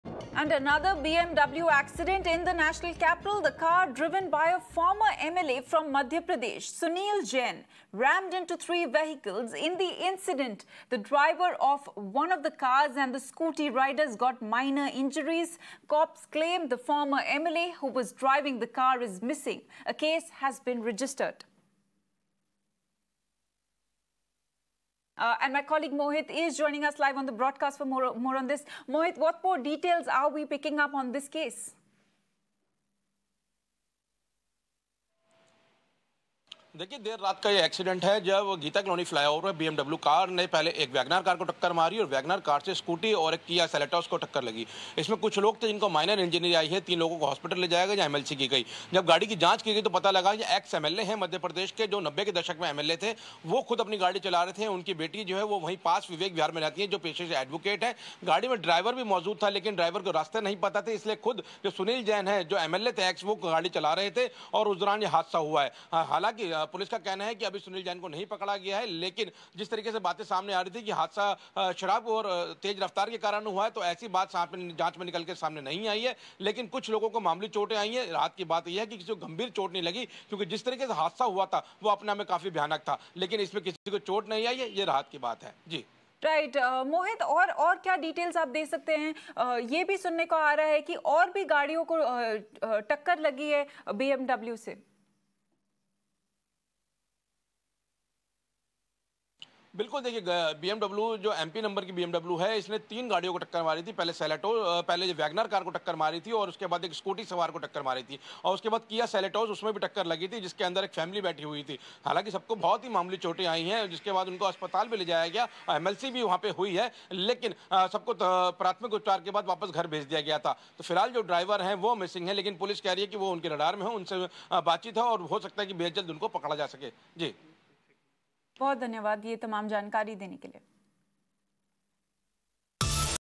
BMW Car Driven By A Former MLA From Madhya Pradesh Sunil Jain, Rammed Into 3 Vehicles In Delhi
As per the latest news on Mirror Now, another BMW accident in the national capital. The car was driven by a former MLA from Madhya Pradesh. Sunil Jain rammed into three vehicles in the incident. The drivers of one of the cars and the scooty riders got minor injuries. Cops claim that the former MLA who was driving the car is missing. A case has been registered.